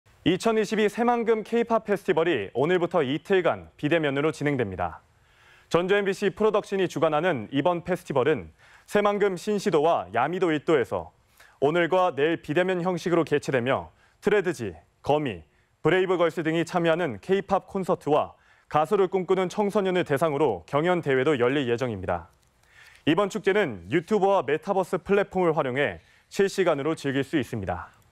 2022 새만금 K-POP 페스티벌, 내일(27일)까지 비대면 개최 | 전주MBC 220826 방송
정자형 기자의 더 많은 기사를 보고싶다면?

2022 새만금 K-POP 페스티벌이 오늘부터 이틀간 비대면으로 진행됩니다.

전주MBC 프로덕션이 주관하는 이번 페스티벌은 새만금 신시도와 야미도 일대에서 오늘(26일)과 내일 비대면 형식으로 개최되며, 트레드지·거미·브레이브걸스 등이 참여하는 K-POP콘서트와 가수를 꿈꾸는 청소년을 대상으로 경연대회도 열릴 예정입니다.

이번 축제는 유튜브와 메타버스 플랫폼을 활용해 실시간으로 즐길 수 있습니다.